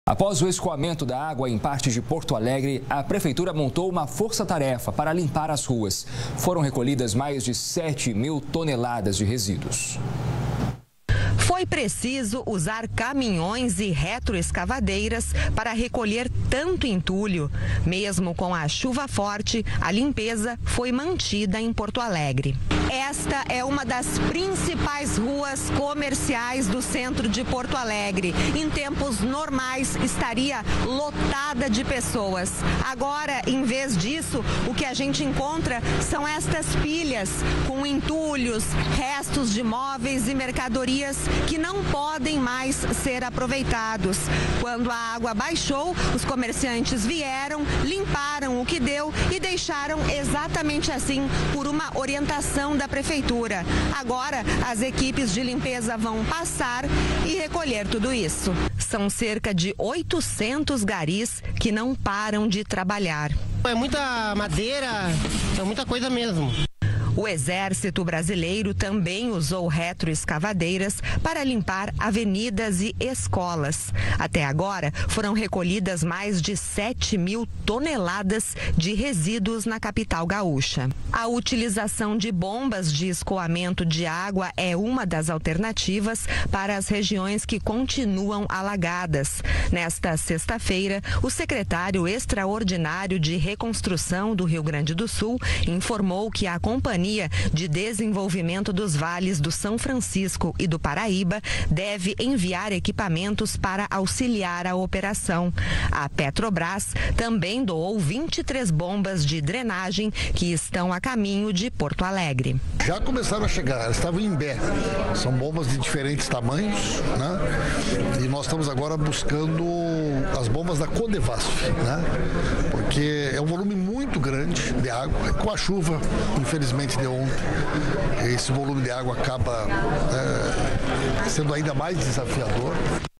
Força-tarefa recolhe mais de sete mil toneladas de resíduos no RS
Após o escoamento da água em parte de Porto Alegre, a prefeitura montou uma força-tarefa para limpar as ruas. Foram recolhidas mais de sete mil toneladas de resíduos.